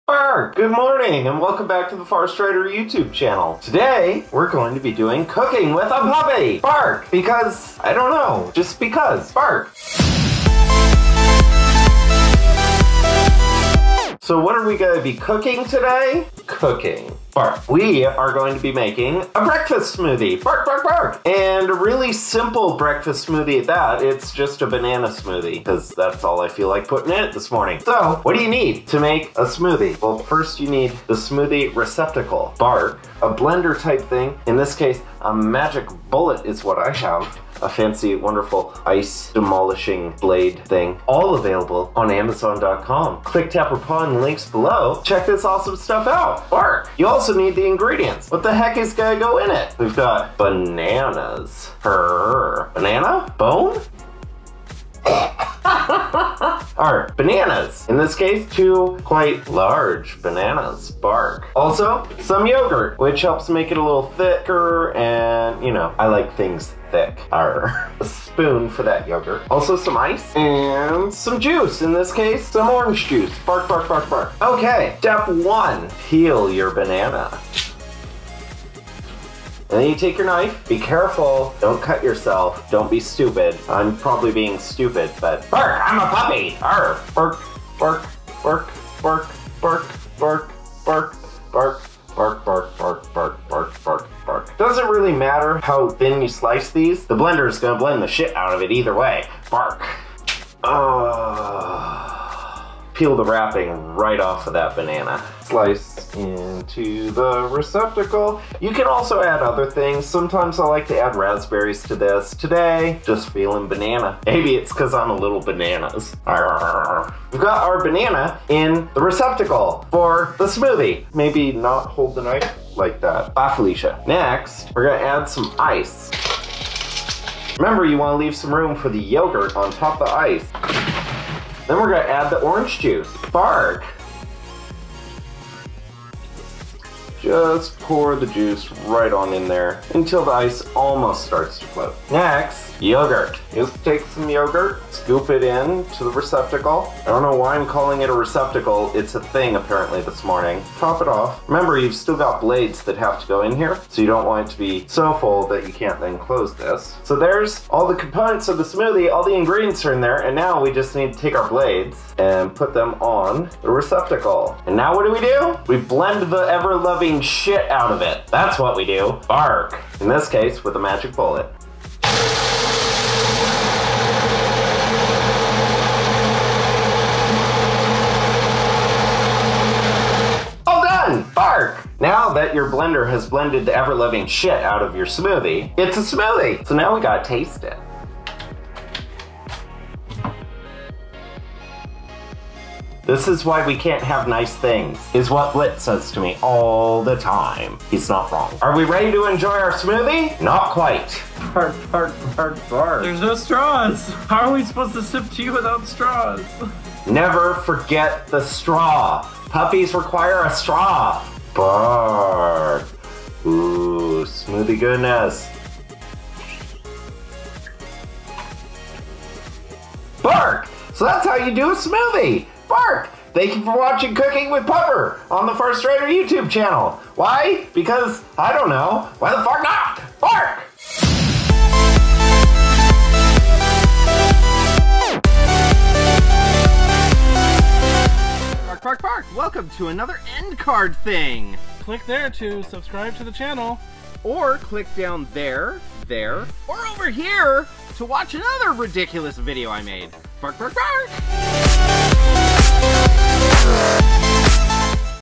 Cooking with Pupper - How to Make a Breakfast Smoothie
Cooking with Pupper: Episode 01 - How to Make a Breakfast Smoothie
In the pilot episode of Cooking with Pupper, Farrstrider makes a ridiculously bannanariffic smoothie... because... obviously! Awrooooo! *wags*

Magic Bullet
Ice Blade
Large Receptacle
Large Receptacle with Lid

Human PuppyKinkstercookingwithpupperkinkyFetishPuppy PlayPup FarrstriderPup Play